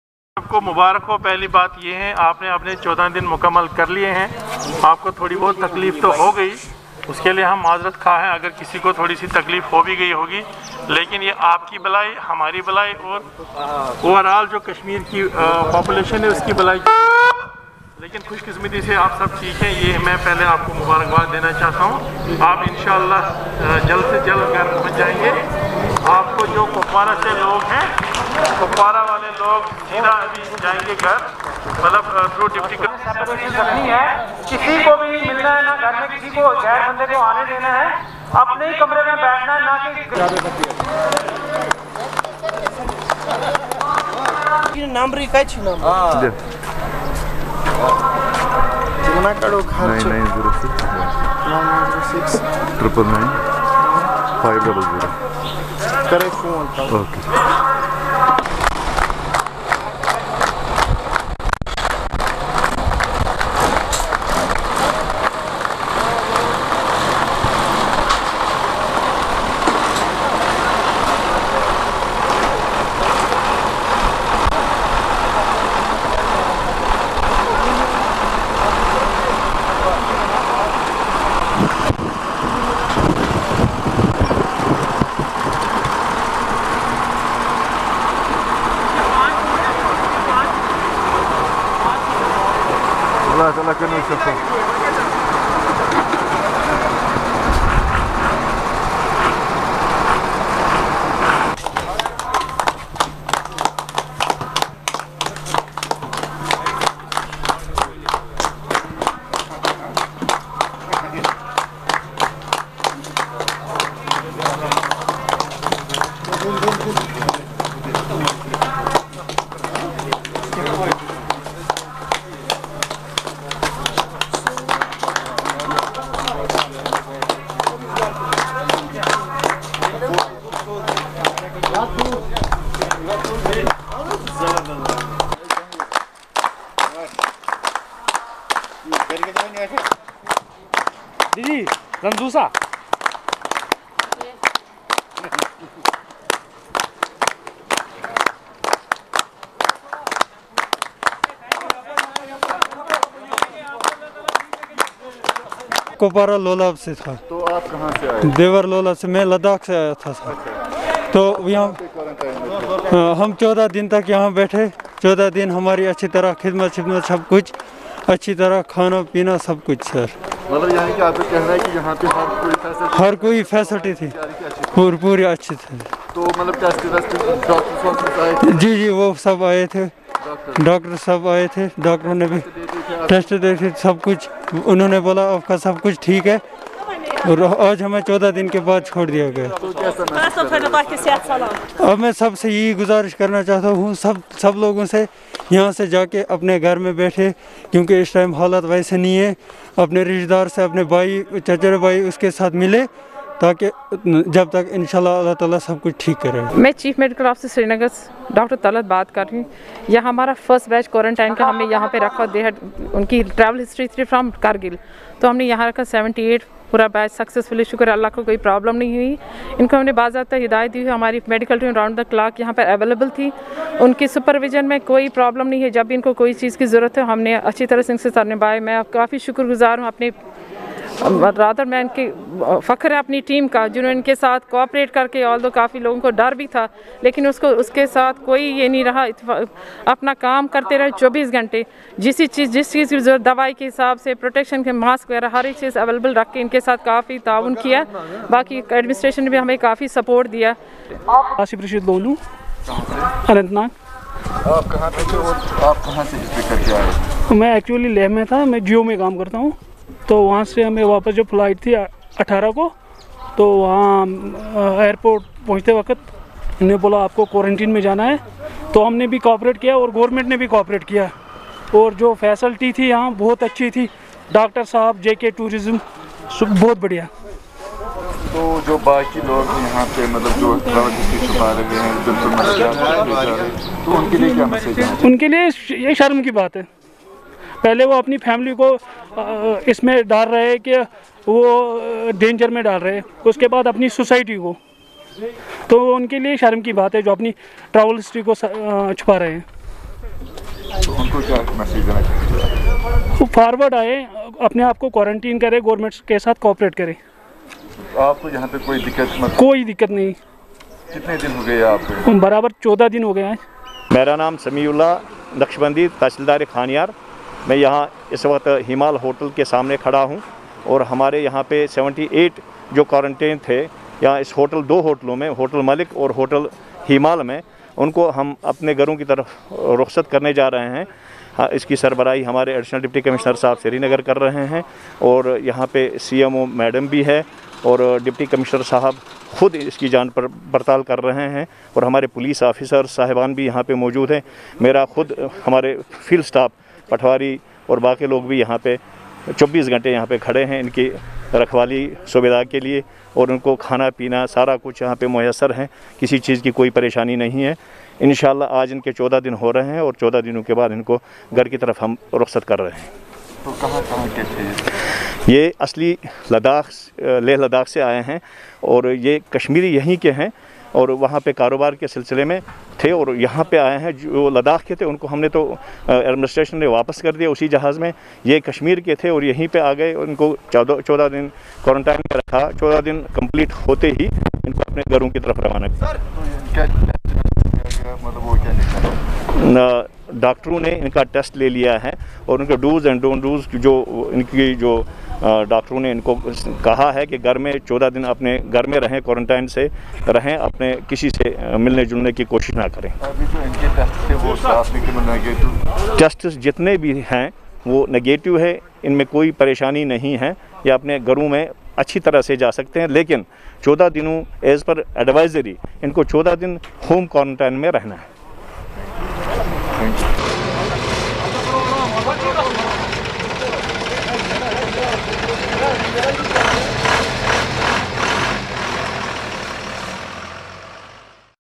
78 people who completed quarantine period after returning from Leh walk home ...
78 people who completed quarantine period after returning from Leh walk home

They were part of first successful quarantine batch: ADC Sgr